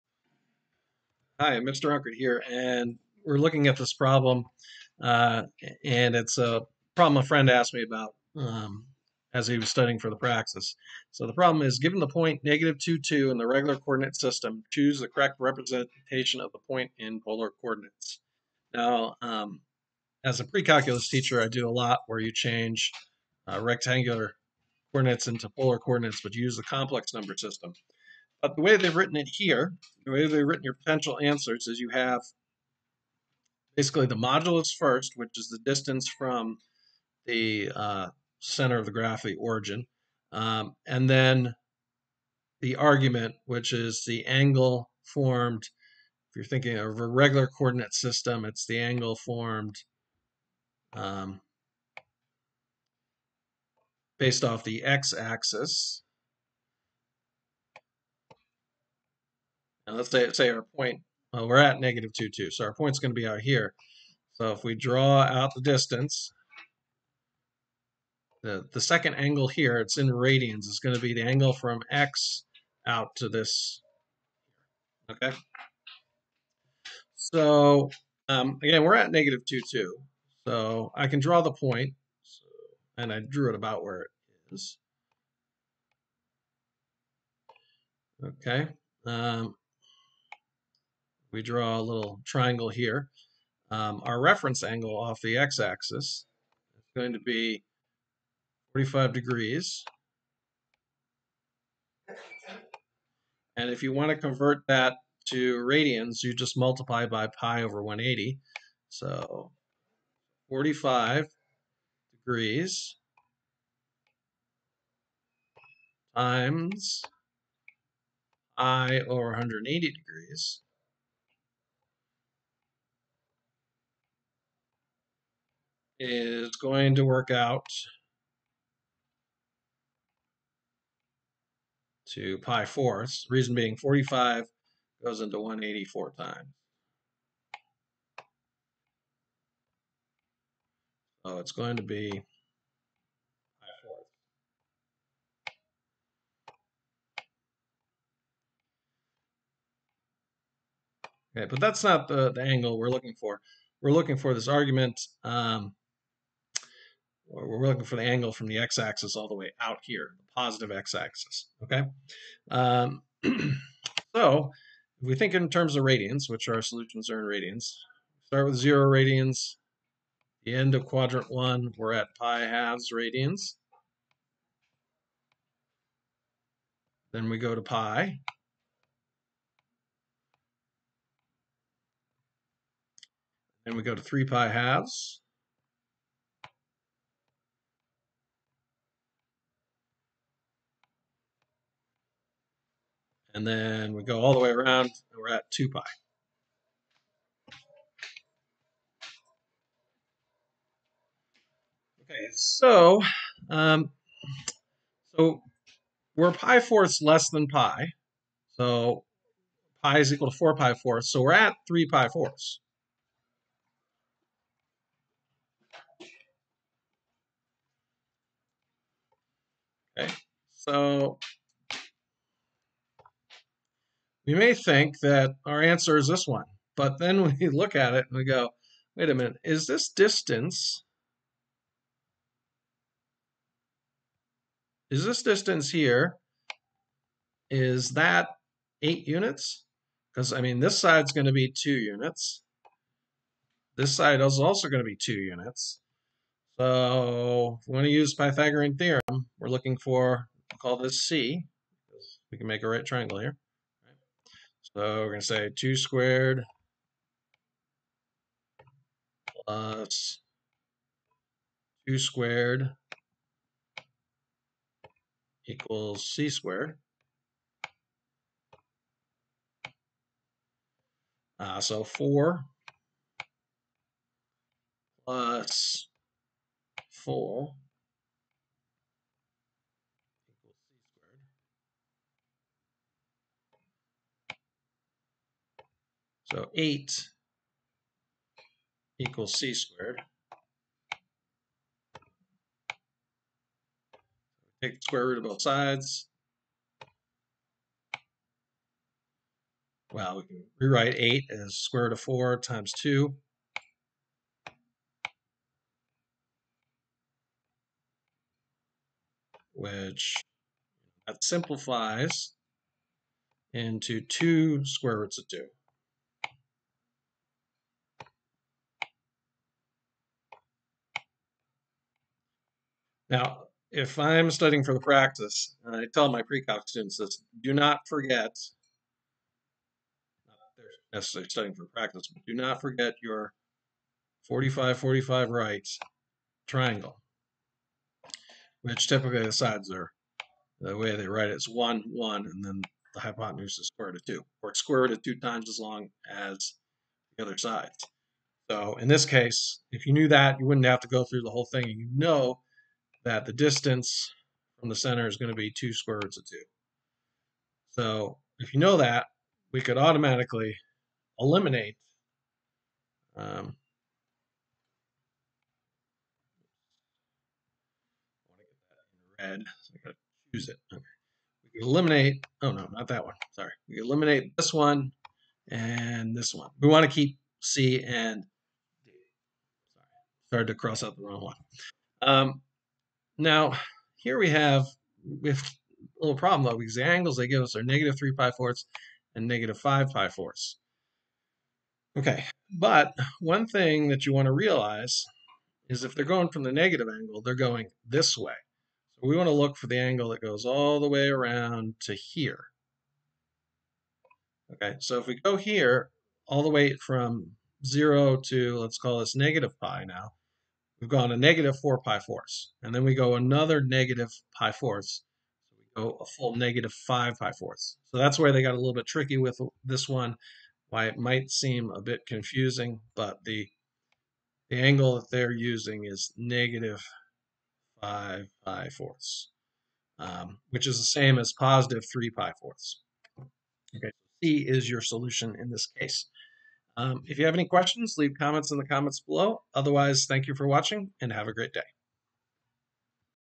High School Math Praxis II (5161) Review - Rectangular to Polar Coordinates
In this video I show you how to switch from rectangular to polar coordinates.